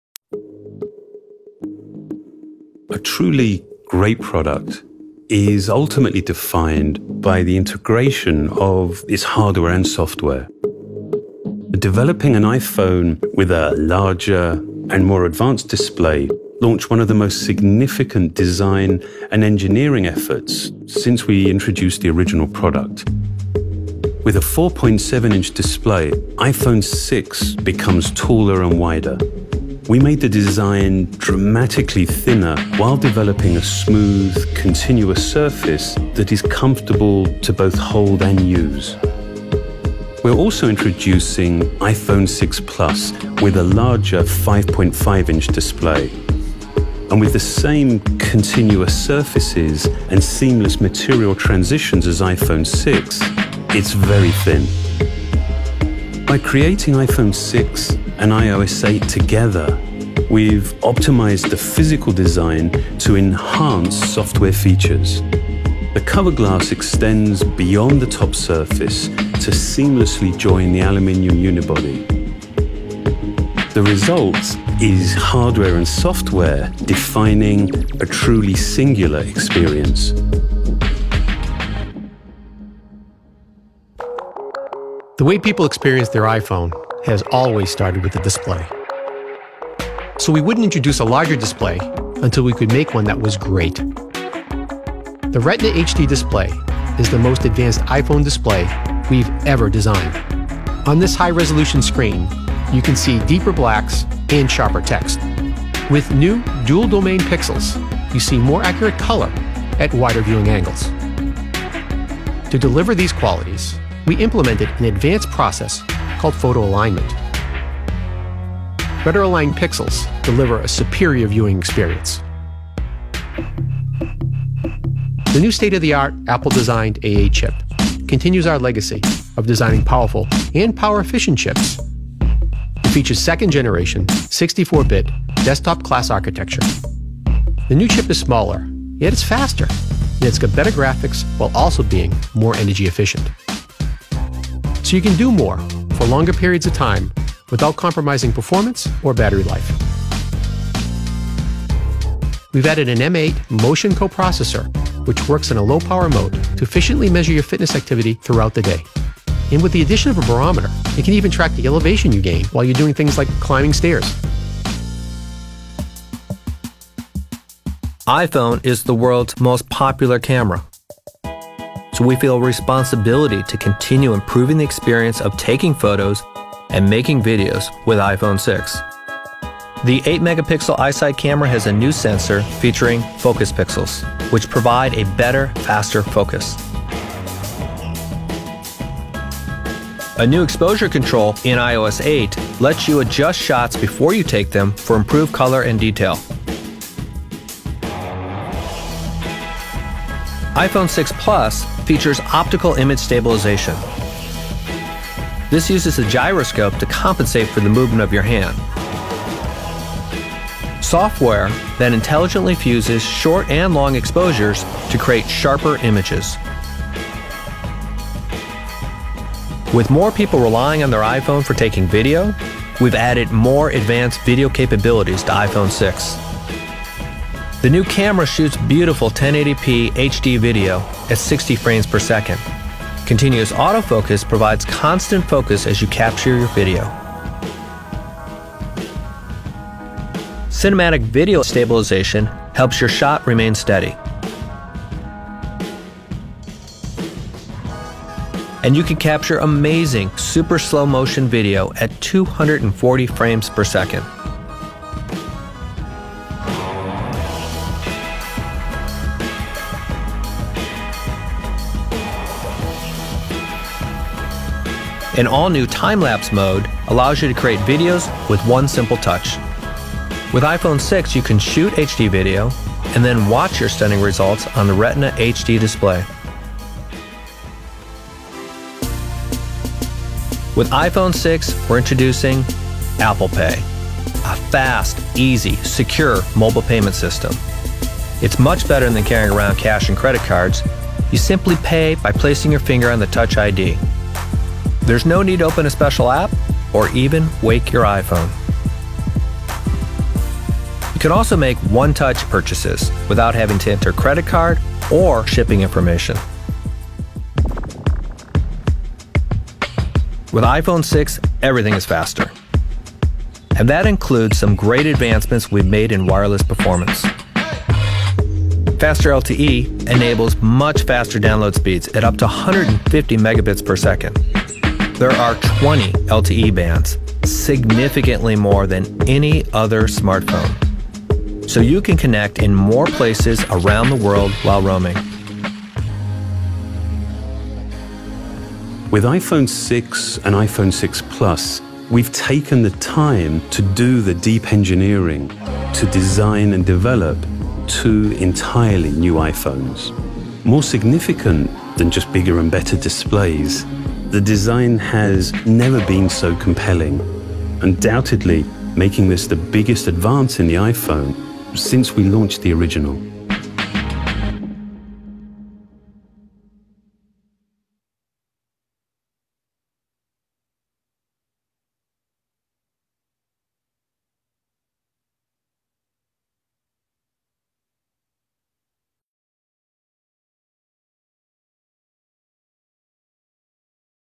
Así son los nuevos iPhone 6 y 6 Plus
Apple presenta sus nuevos modelos de iPhone 6 y iPhone 6 Plus, que adoptan el formato phablets.